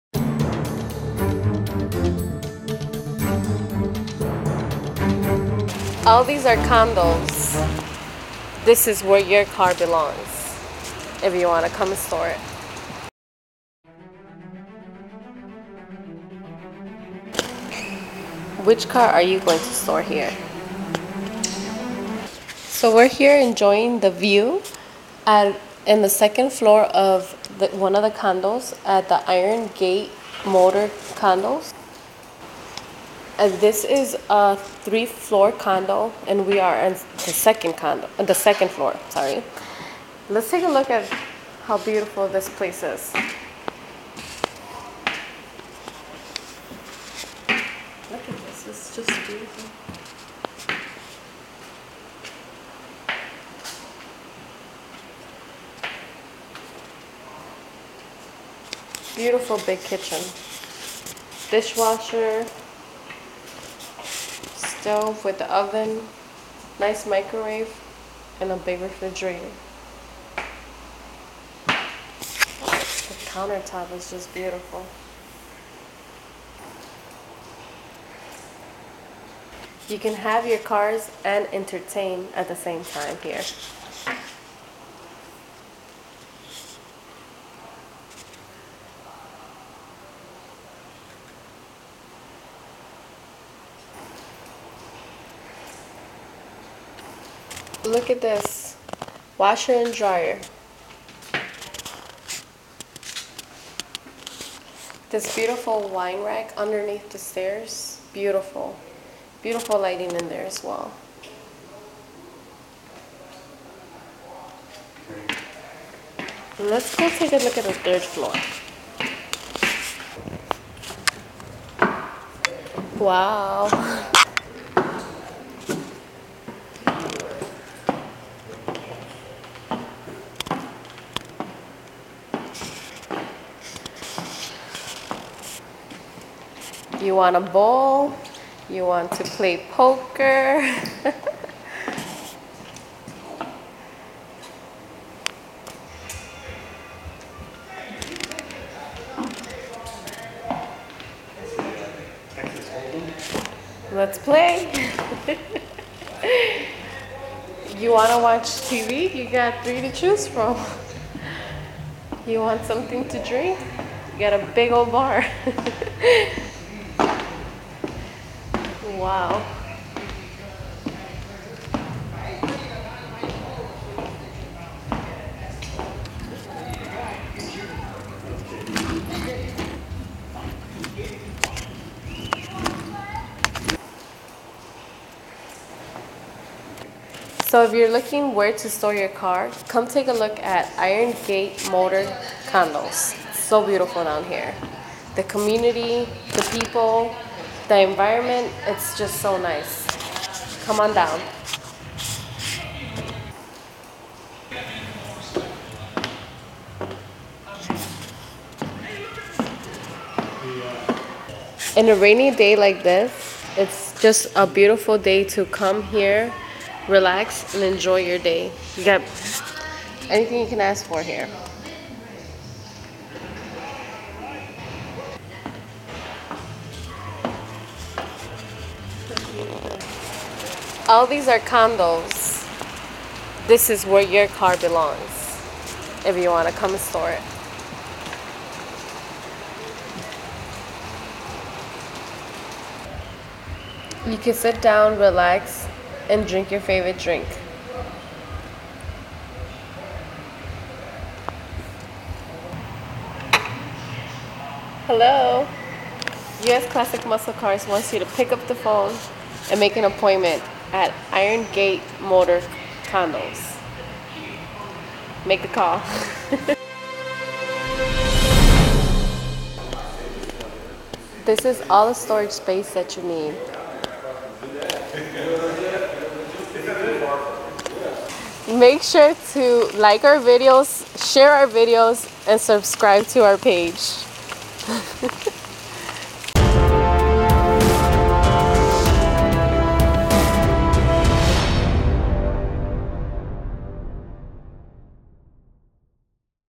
INCREDIBLE CONDO FOR YOUR CAR- Irongate Motor Condos for Supercars, Muscle Cars and Your Car
USClassicMuscleCars was given access to one of the Irongate Motor Condos.  The owner said film your best and continued hhis phone call knew USClassicMuscleCars would portray the best life when living the dream.  Irongate Motor Condos is a community near Chicago that combines the best of two worlds, Great Storage of your cars while in a condo environment.   NICE